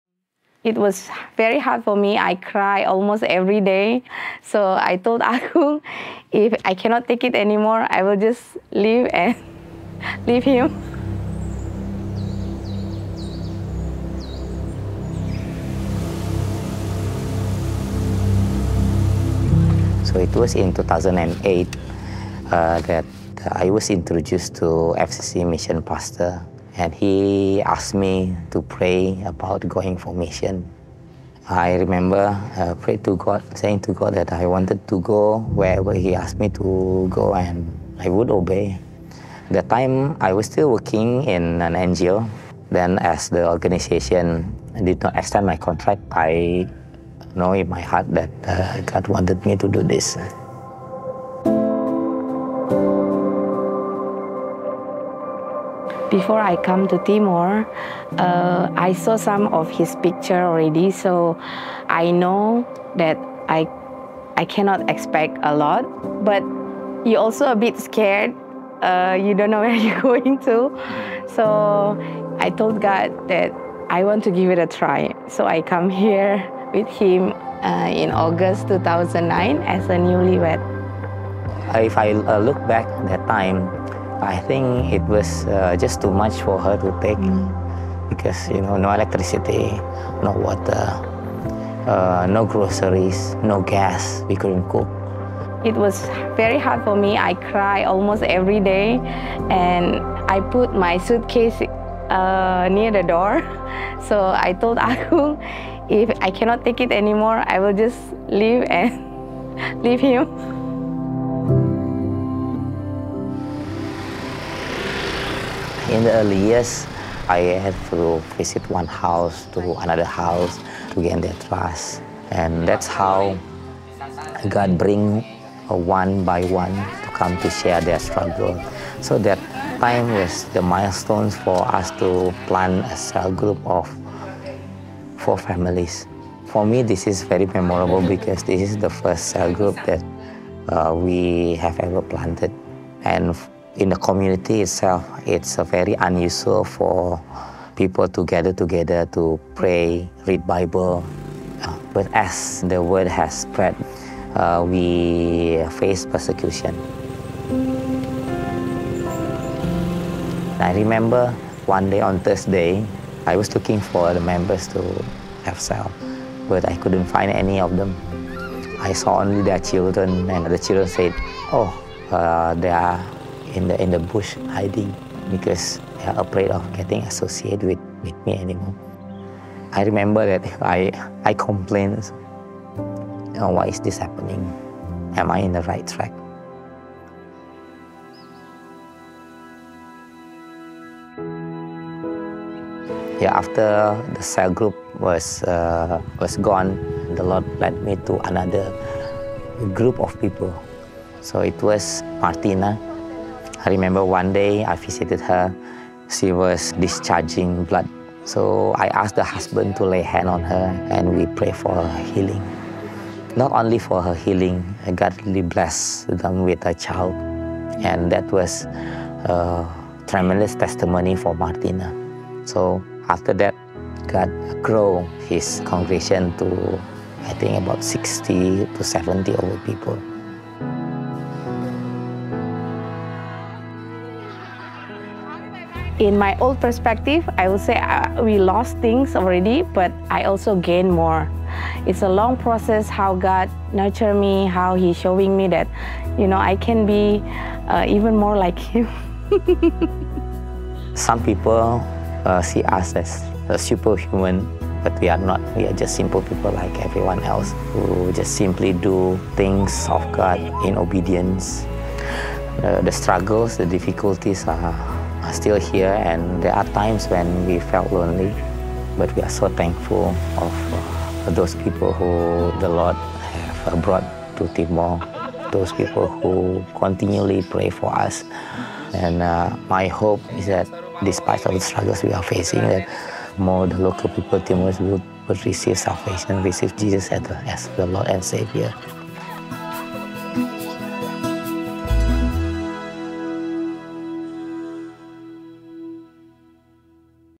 Overcoming Struggles and Persecution: A Missionary's Journey
Hear from Agung, our missionary in East Timor, and his wife Andry, as they share their journey of leaving their home countries to answer God's call. Despite the struggles and persecution they’ve faced, they continued to seek His Kingdom first, trusting the Lord every step of the way.

We are deeply grateful for their faithful service and pray that the Lord continues to bless the works of their hands.